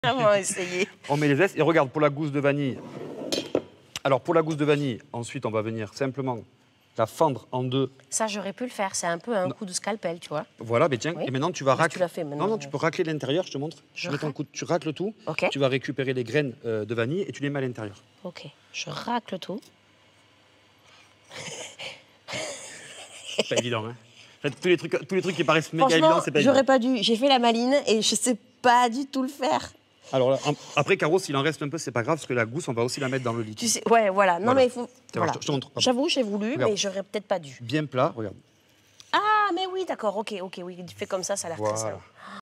La Cuisine des Mousquetaires - Poire Belle-Hélène en robe sauce chocolat piment d'Espelette
Aujourd’hui, place au dessert dans ce quatrième épisode avec la poire Belle-Hélène. Pour cette revisite, le fruit est habillé d’une robe à base de pâte brisée, pour des bouchées fondantes et croquantes, accompagné d’une sauce chocolat.

Un moment plein de gourmandise avec nos deux protagonistes Caroline Estremo et Fabrice Mignot qui vous accompagnent dans la réalisation de cet incontournable de la cuisine.